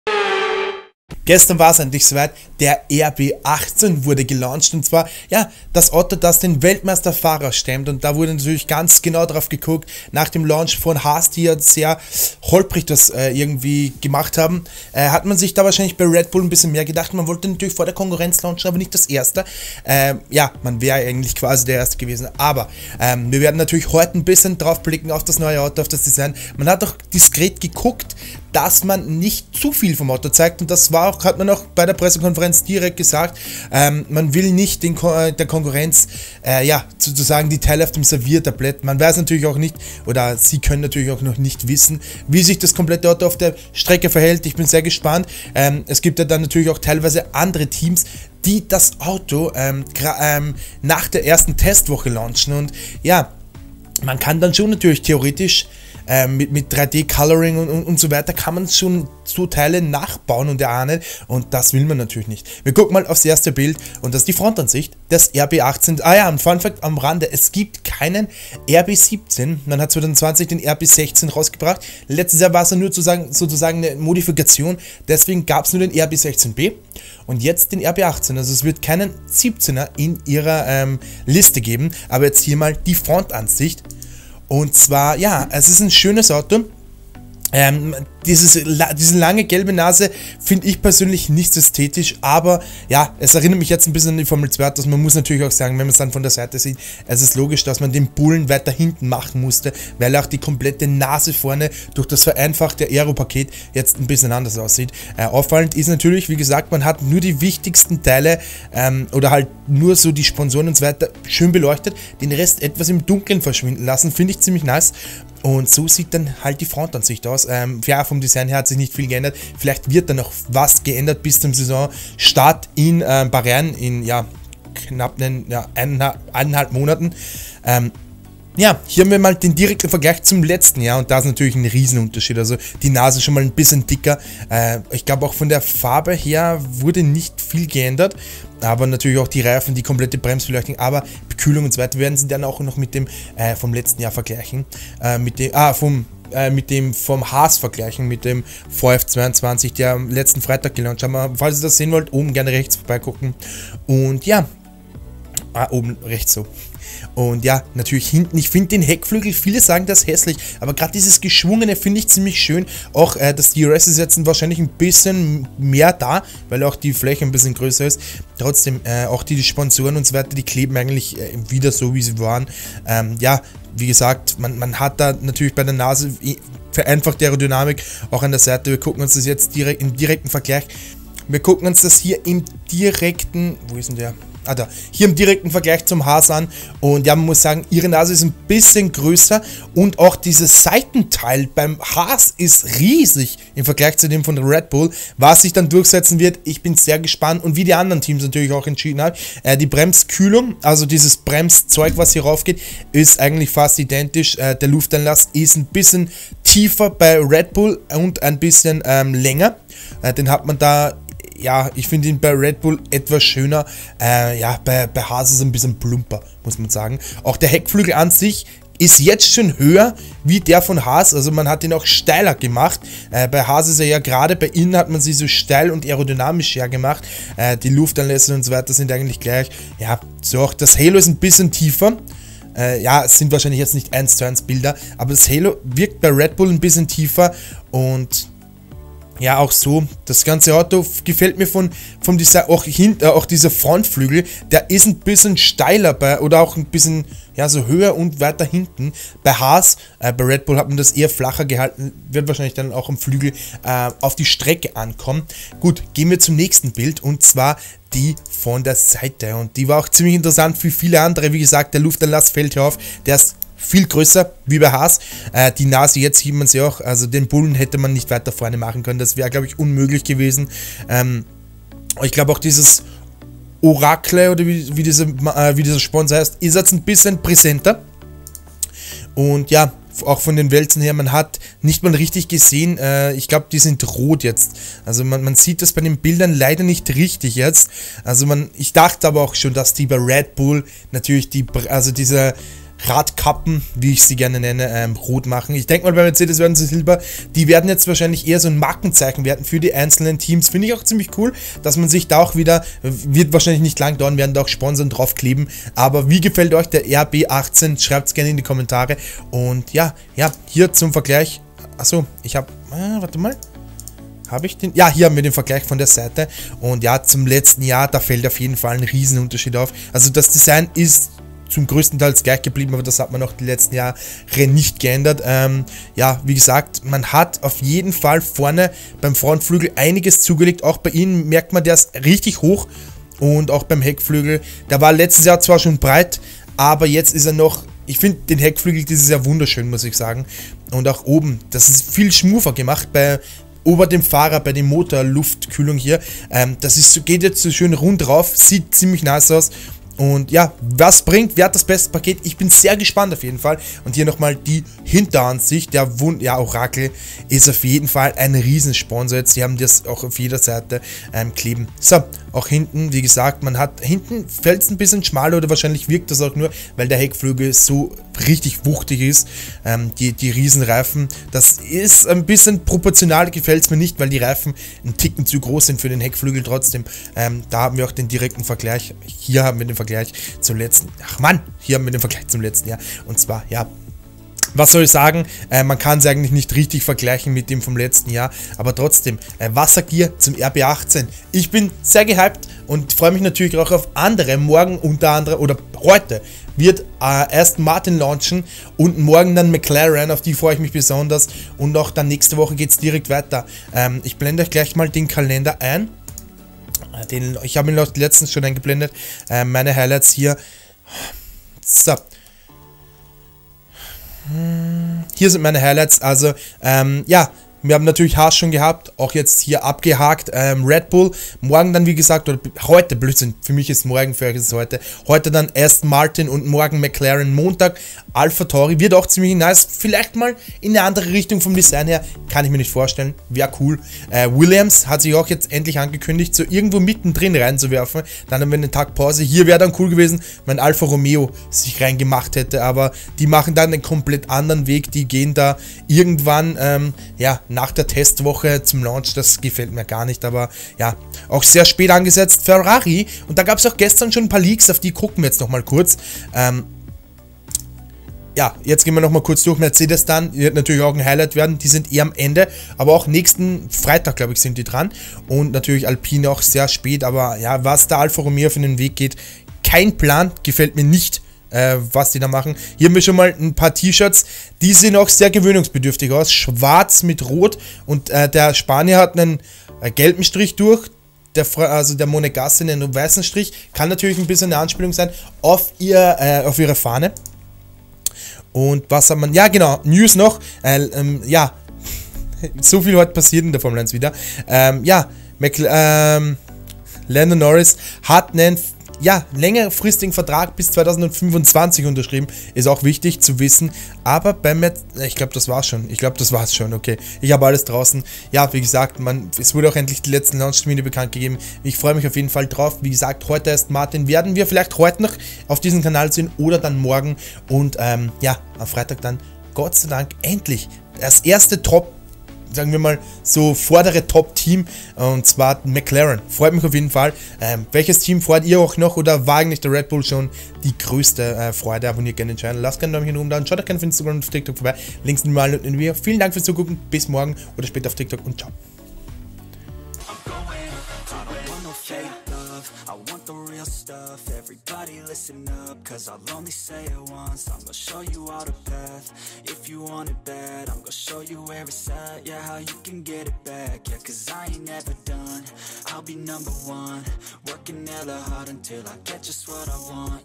REDBULL RB-18 LAUNCH 2022 - F1 Lauches #02
Die NUMMER 1 hat vorgestellt, zumindest das Auto mit der Startnummer 1! Alle NEWS, BILDER und VERGLEICHE zum RB-18 HIER!